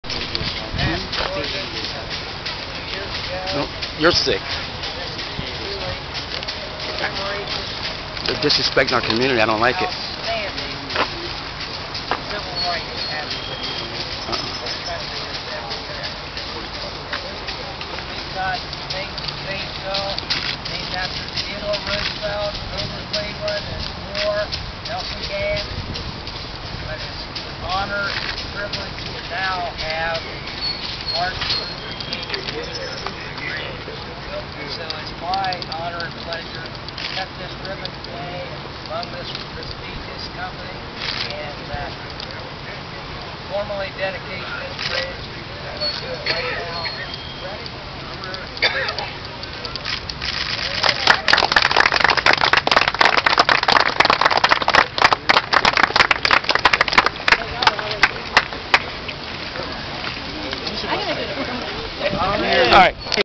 Psychological Slaves
Shame.. Zanesville Ohio... These people are dedicating this bridge, in spite of The Four Little Girls who passed a Lawful Petition to create it (June 2009).